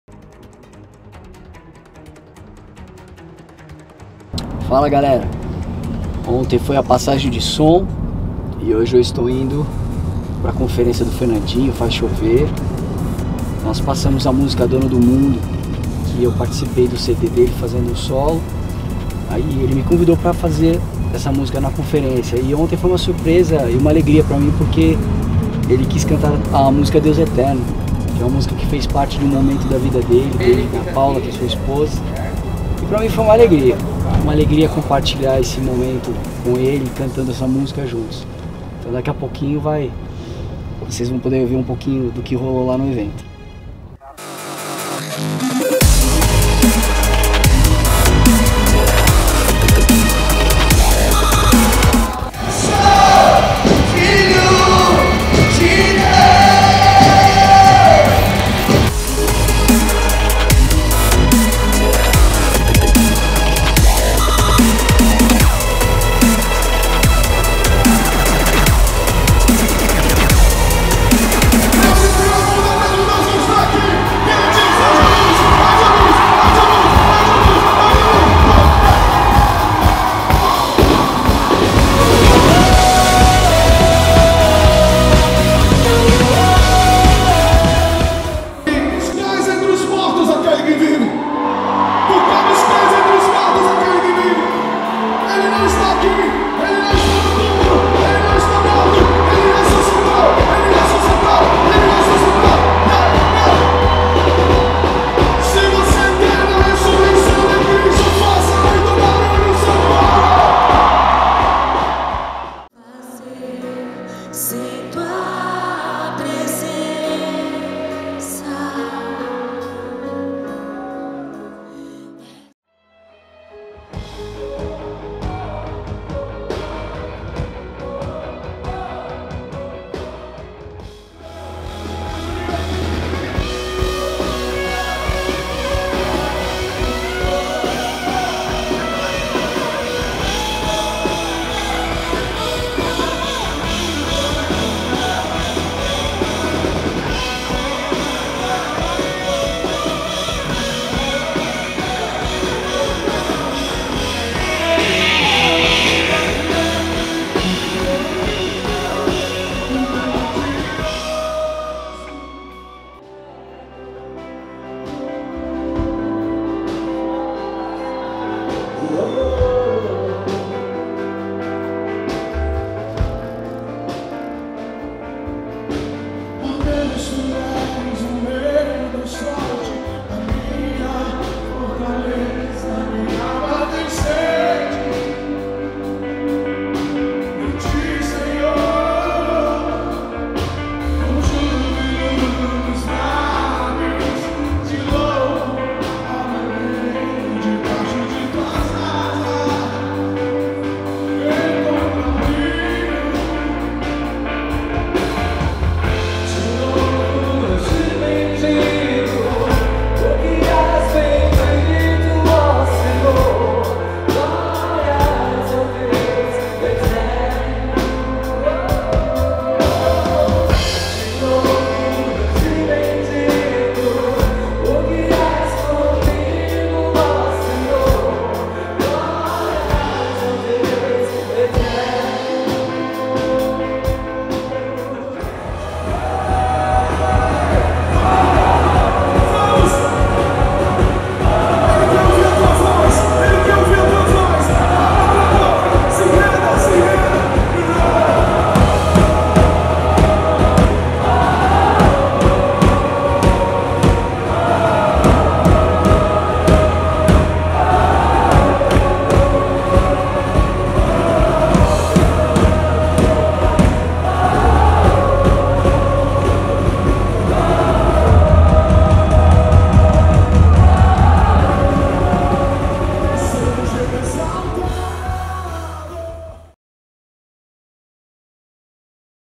VLOG - Juninho Afram | Fernandinho - Conferência "Faz Chover"
Fala Galera!! Nesse vlog vcs vão ver um pouco do que rolou na conferência Faz Chover do Fernandinho em SP. Espero que gostem...
Abssssss